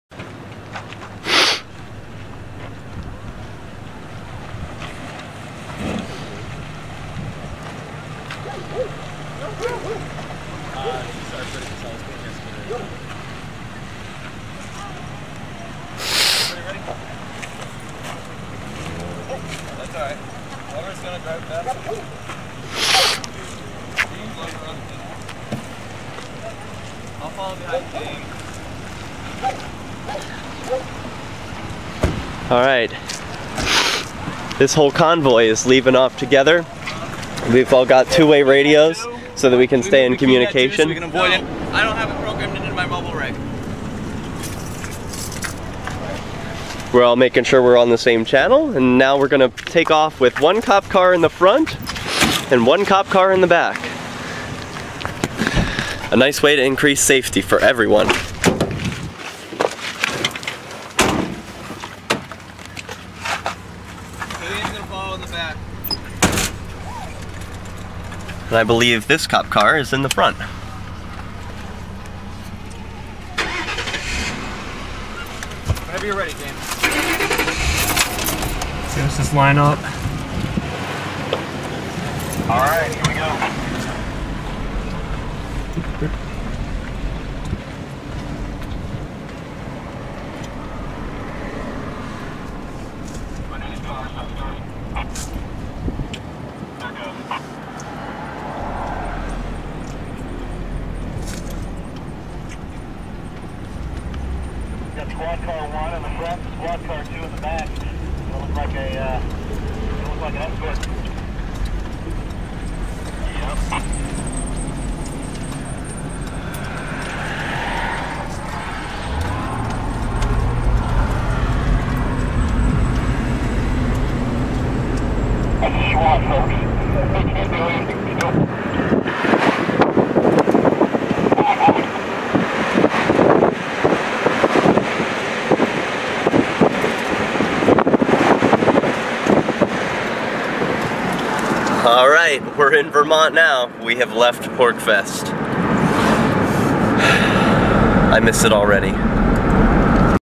Caravan Leaves the Station | PorcFest Derrick Cam Day 8
Sunday, June 29th 2014 - The Porcupine Freedom Festival in the White Mountains of New Hampshire. Vendors and vacationers gather from all around to serve one another, listen to speeches, and connect with others who want to get active for liberty in their lifetime.